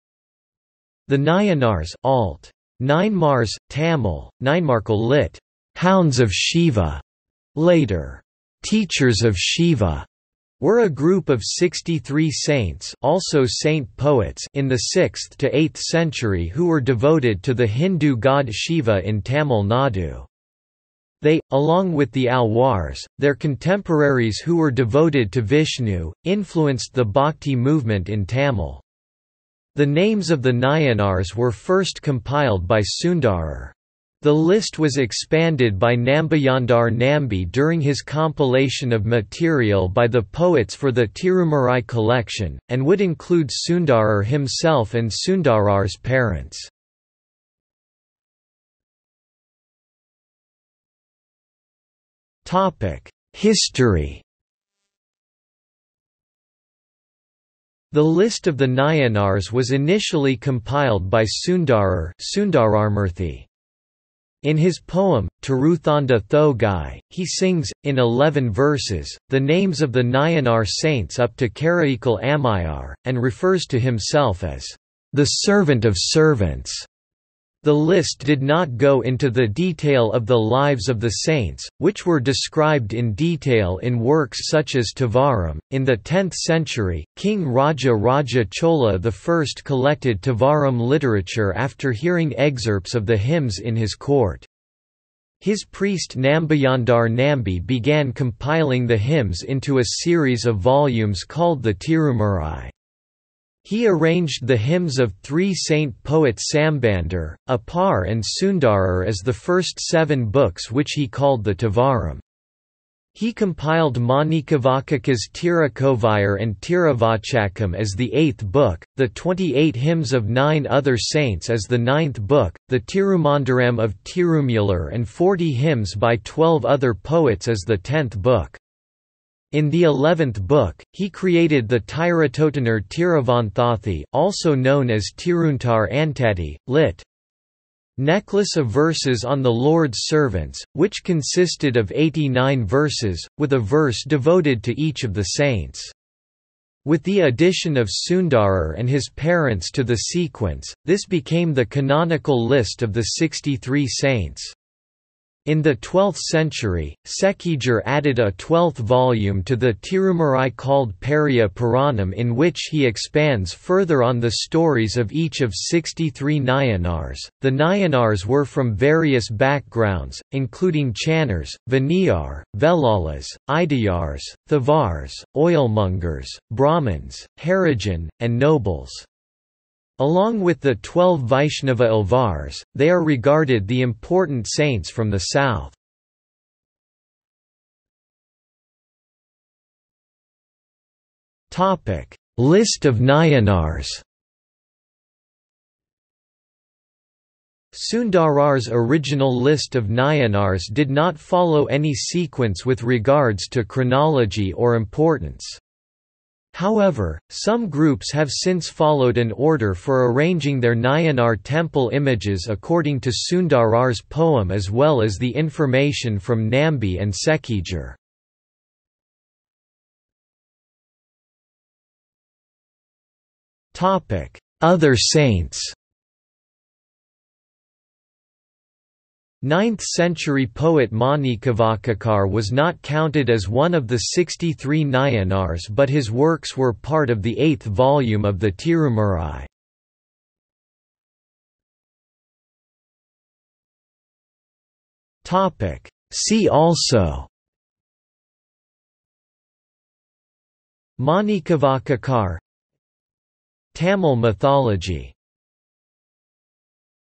Nayanars | Wikipedia audio article
00:00:48 1 History
00:03:07 2 List of Nayanars
00:03:40 3 Other saints
00:04:01 4 See also

- Socrates

SUMMARY
The Nayanars (alt. Nayanmars, Tamil: நாயன்மார்கள், lit. "hounds of Siva", later "teachers of Siva") were a group of 63 saints (also saint poets) in the 6th to 8th century who were devoted to the Hindu god Shiva in Tamil Nadu. They, along with the Alwars, their contemporaries who were devoted to Vishnu, influenced the Bhakti movement in Tamil. The names of the Nayanars were first compiled by Sundarar. The list was expanded by Nambiyandar Nambi during his compilation of material by the poets for the Tirumurai collection, and would include Sundarar himself and Sundarar's parents.